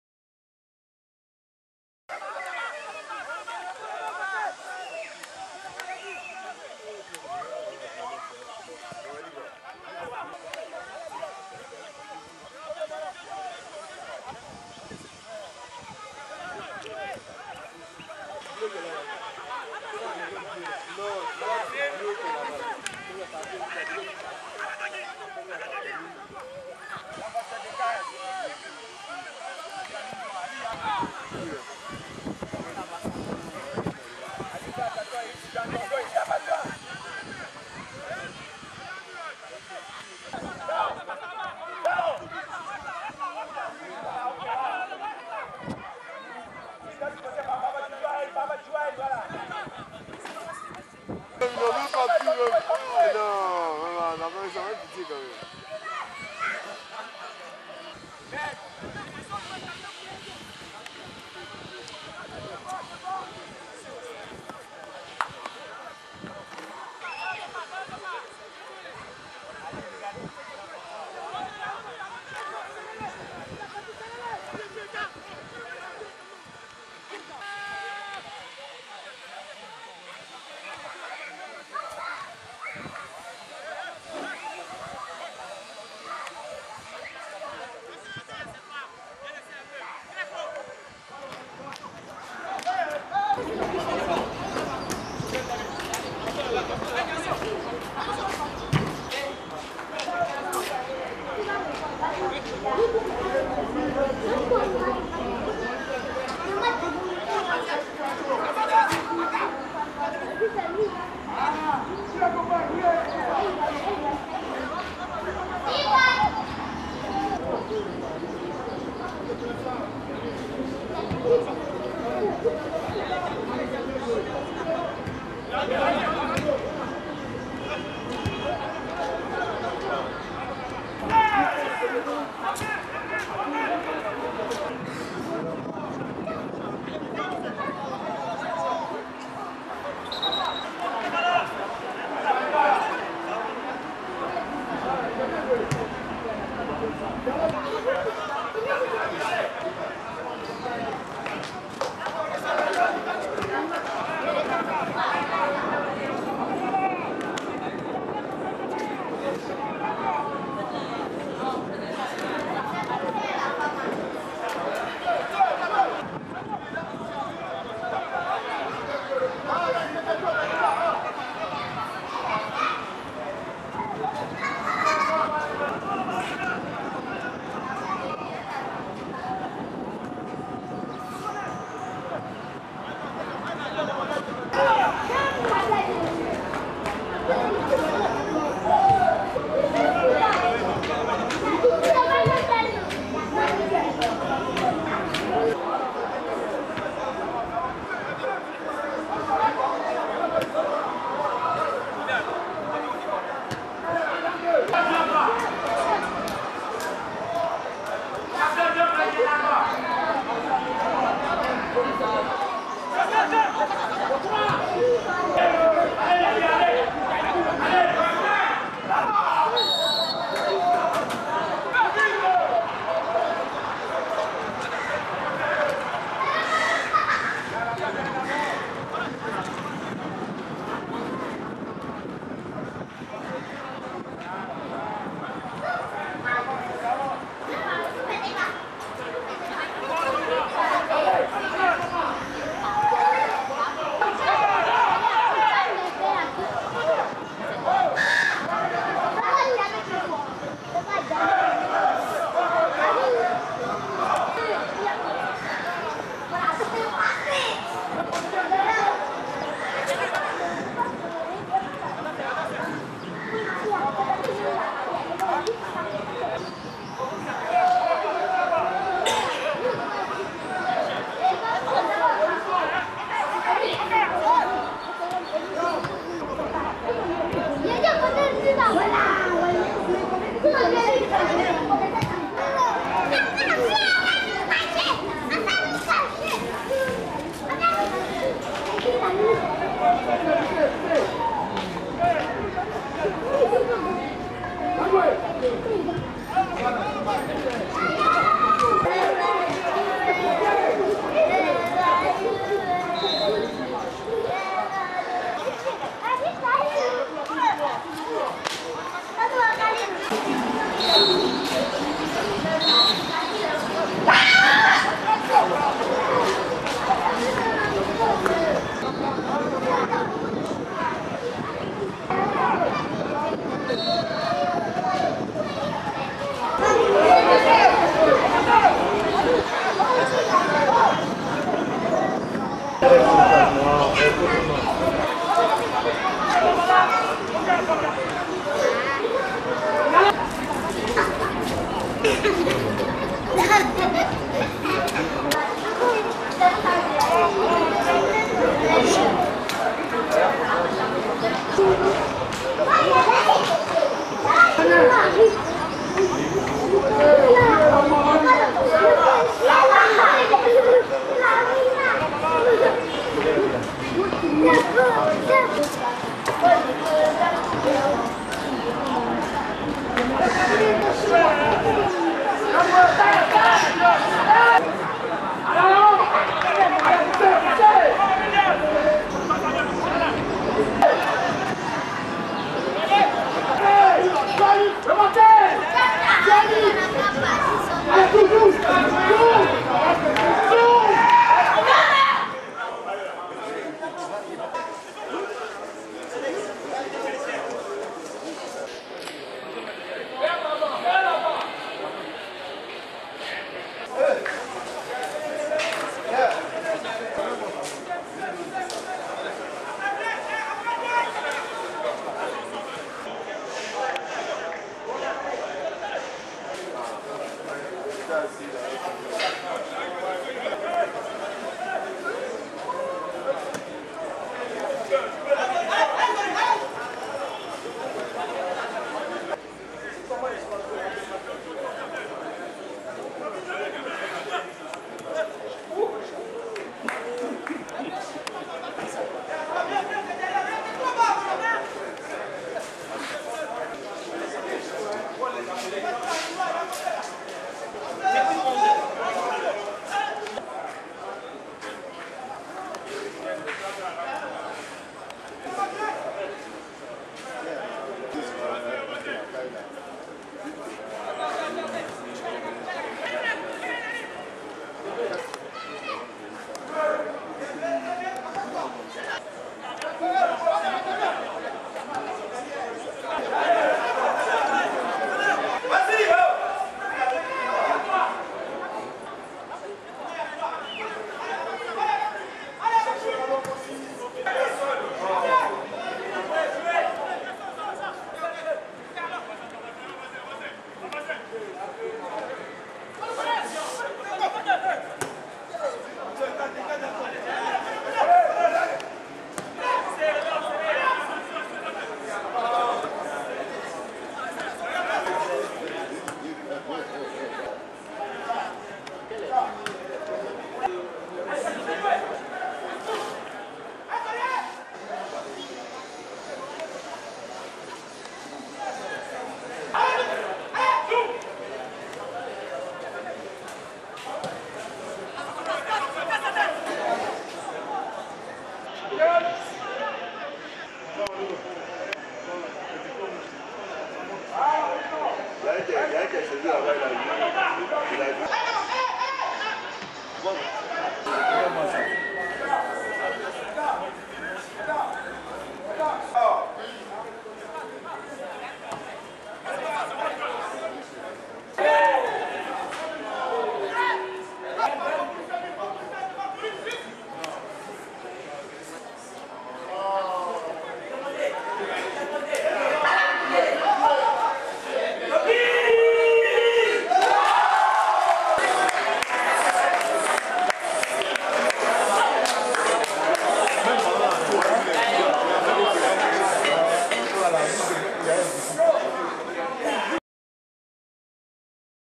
Prestation KONE IBRAHIM .
KONE IBRAHIM dans différentes prestations avec ES BINGERVILLE, l'equipe dont il est le capitaine.